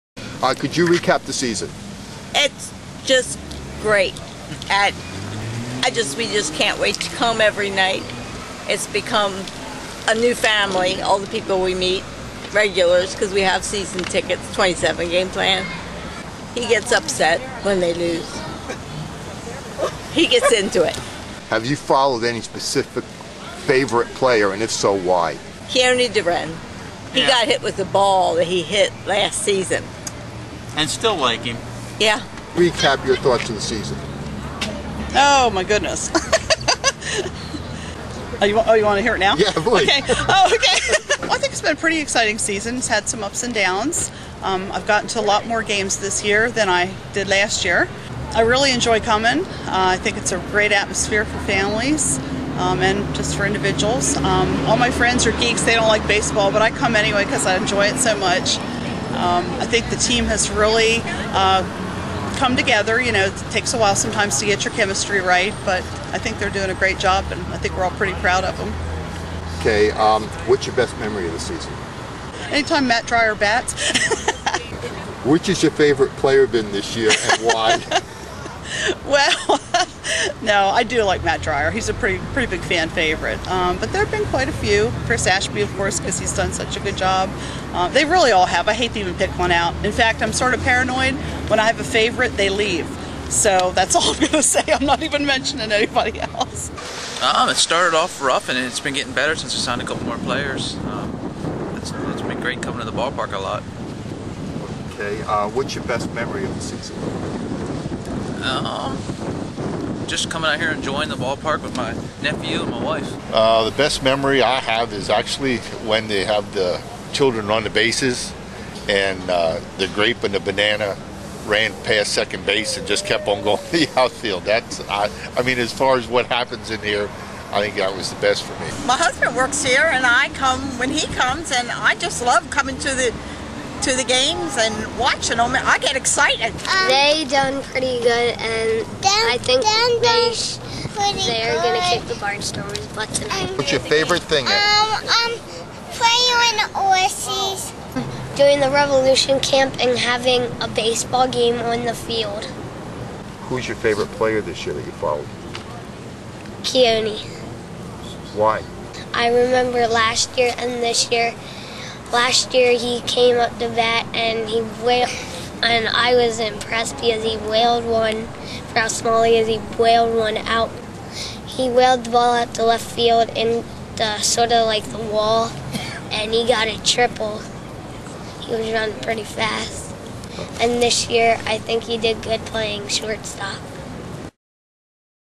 Rev fans talk
York Revolution fans talk about the season and the team.